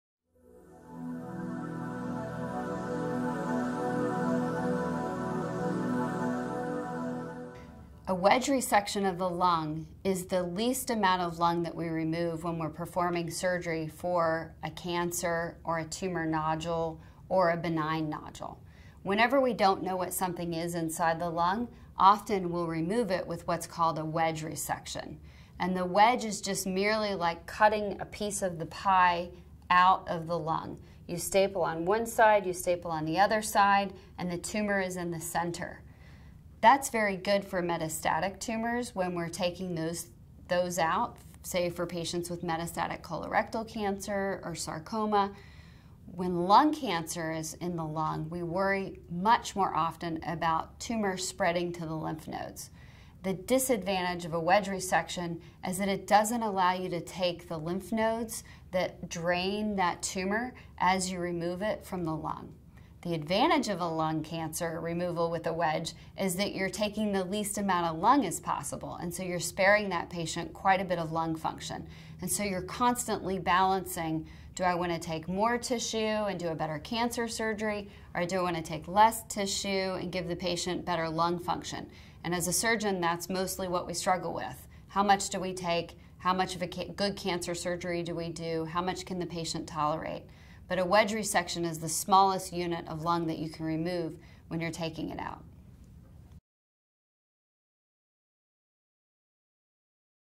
What is a wedge surgery?
A wedge surgery or wedge resection of the lung is the least amount of lung that a doctor will remove when performing surgery for a cancer or a tumor nodule or a benign nodule.